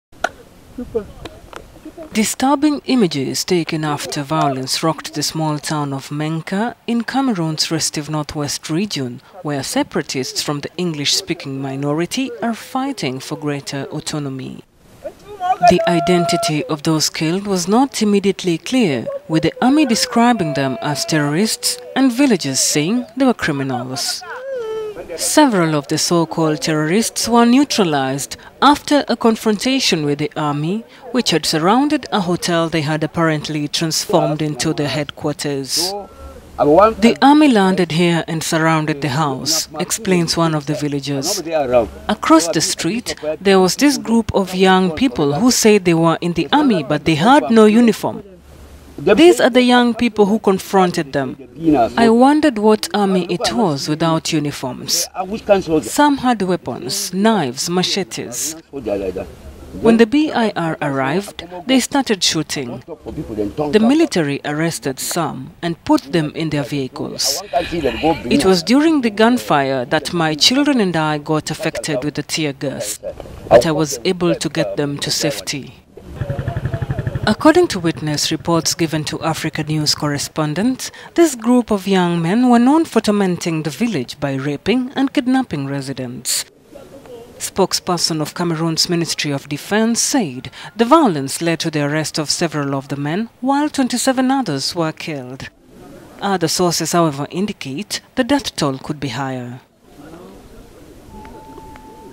Cameroon: Dozens of separatists killed in Menka
Disturbing images taken after violence rocked the small town of Menka, in Cameroon's restive north-west region, where separatists from the English-speaking minority are fighting for greater autonomy.

The identity of those killed was not immediately clear, with the army describing them as "terrorists" and villagers saying they were criminals.

Several of the so-called terrorists were neutralized after a confrontation with the army which had surrounded a hotel they had apparently transformed into…